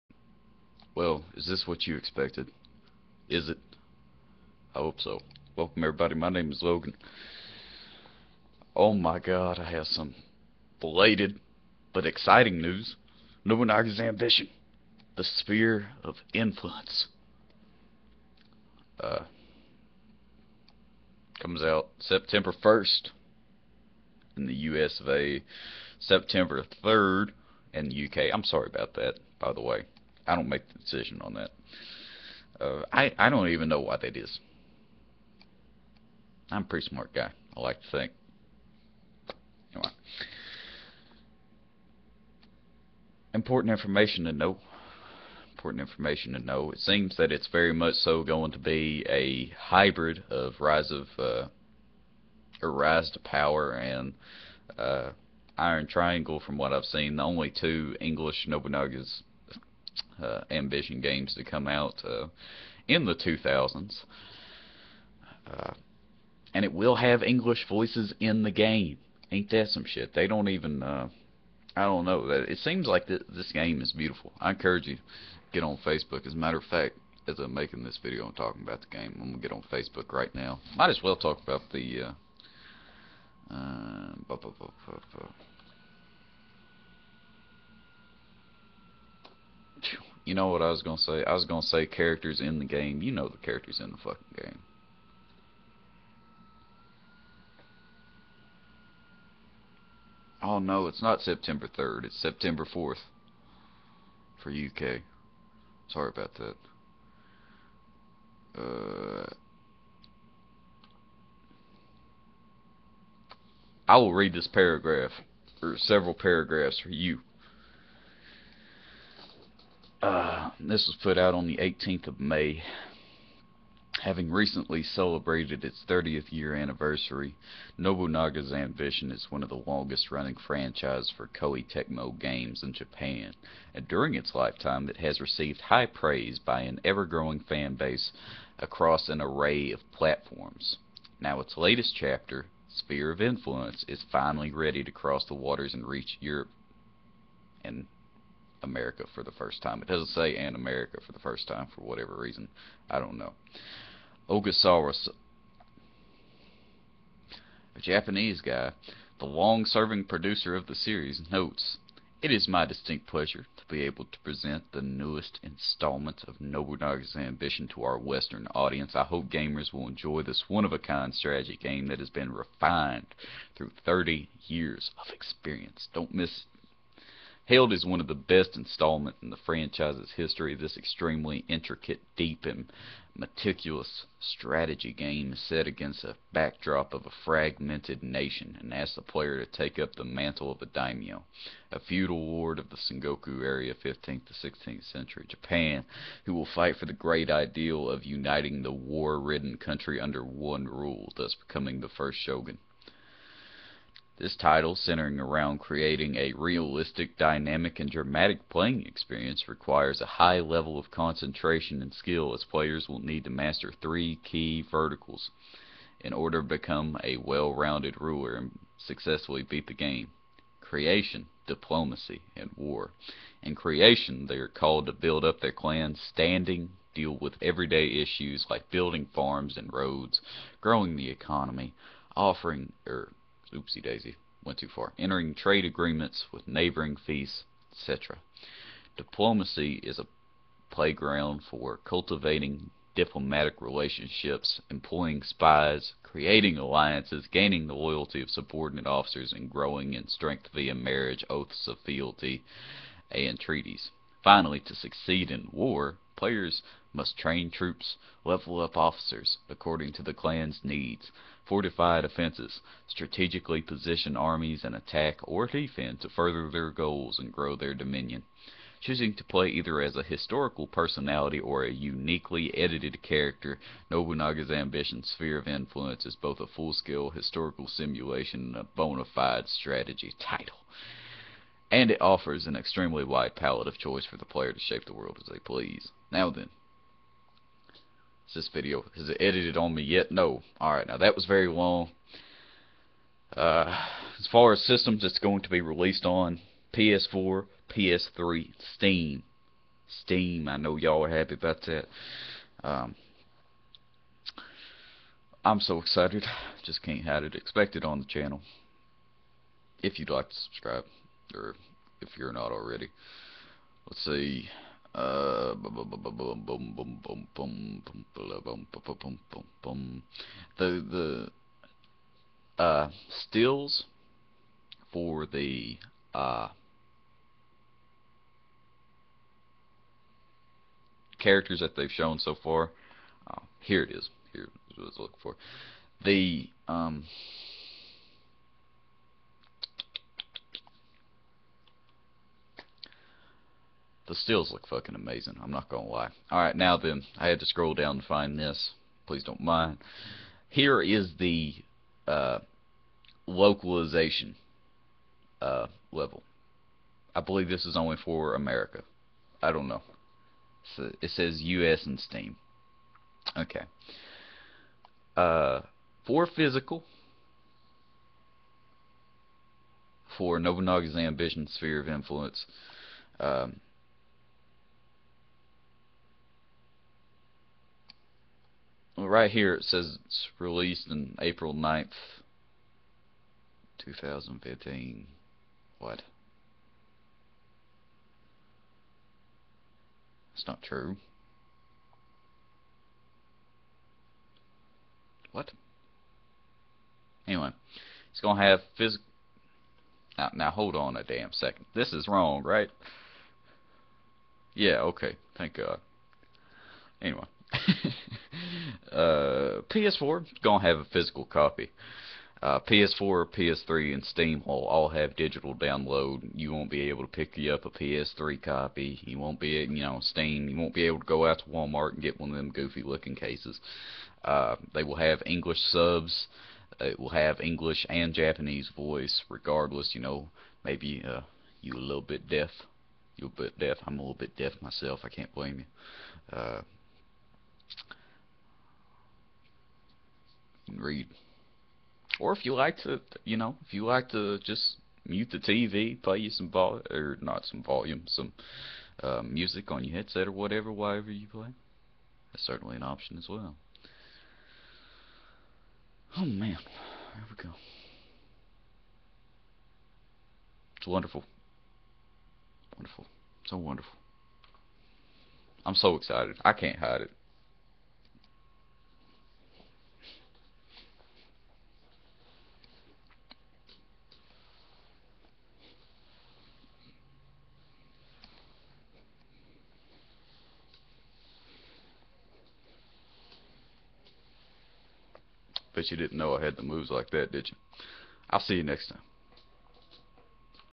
'Nobunaga's Ambition: Sphere of Influence' release dates and such
Can't wait to try this shit out. For those of you who just want release dates.
9/1/2015 for the US.
9/4/2015 for the UK.